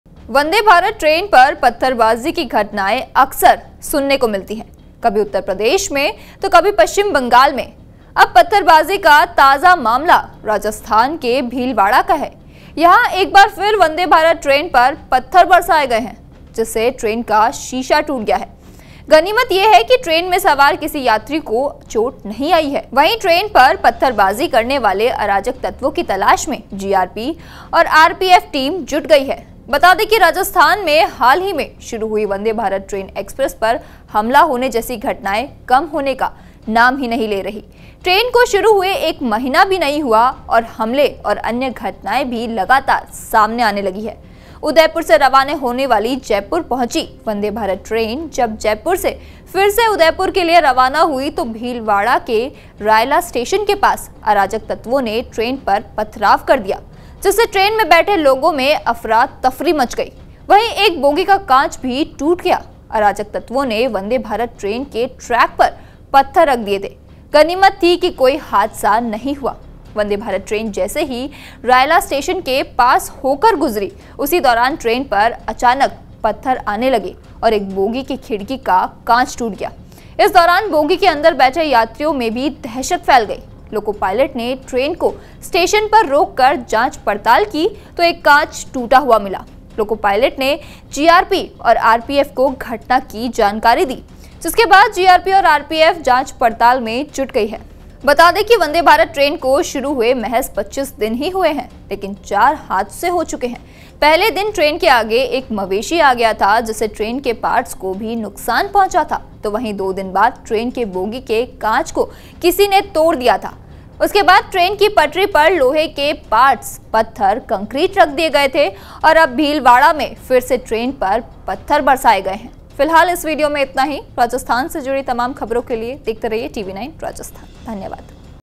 Rajasthan में वंदे भारत पर फिर पथराव, Bhilwara में बरसाए पत्थर, टूटा खिड़की का शीशा
उदयपुर से जयपुर के लिए शुरू हुई वंदे भारत ट्रेन पर पथराव की घटना एक बार फिर हुई है. अराजकतत्वों ने ट्रेन पर पथराव कर दिया, जिससे उसकी खिड़की का शीशा टूट गया. घटना बुधवार रात भीलवाड़ा के रायला क्षेत्र में हुई. 25 दिन में इस ट्रेन पर चार हादसे हो चुके हैं.

The incident of stone pelting on Vande Bharat train which started from Udaipur to Jaipur has happened once again. The anarchists pelted stones at the train, due to which its window glass was broken. The incident took place in Rayala area of Bhilwara on Wednesday night. There have been four accidents on this train in 25 days.